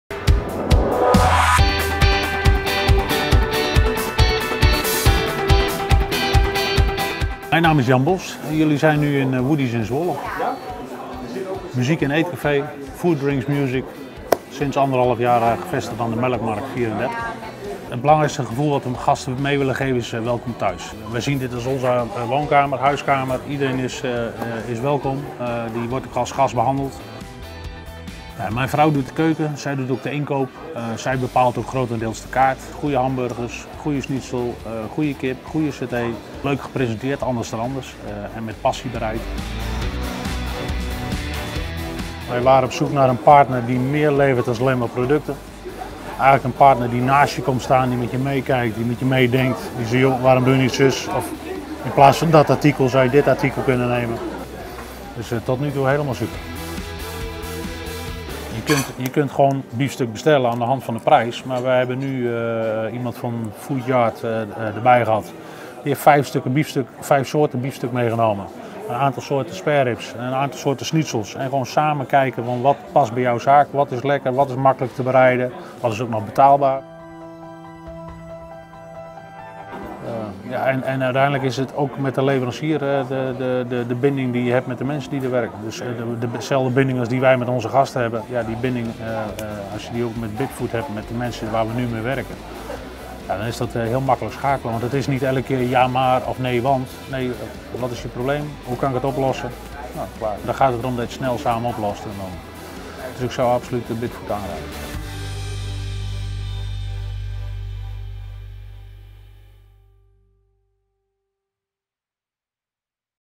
[KLANT-IN-BEELD] Waarom kiest Jan Bos van Woodies uit Zwolle voor Bidfood?
Bidfood helpt Jan Bos van Muziek- en eetcafé Woodies in Zwolle bij zijn onderneming. Een zeer prettige samenwerking waar wij blij mee zijn. Wat voor ervaringen Jan heeft met Bidfood? Check de video!

Bidfood, voorheen Deli XL.